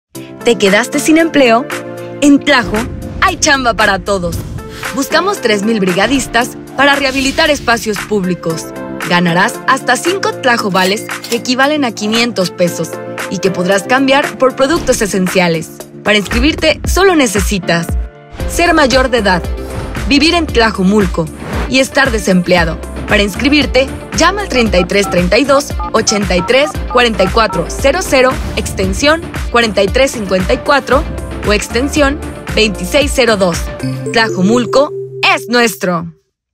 En Tlajo hay Chamba para Todos. Inscríbete como brigadista - Gobierno de Tlajomulco
👷‍♂️👷‍♀️ Estamos buscando 3,000 brigadistas para que se inscriban al programa Chamba para Todos en el que podrán obtener hasta 5 Tlajovales, equivalentes a 500 pesos. Aquí te decimos qué necesitas y el número al que te puedes comunicar para inscribirte.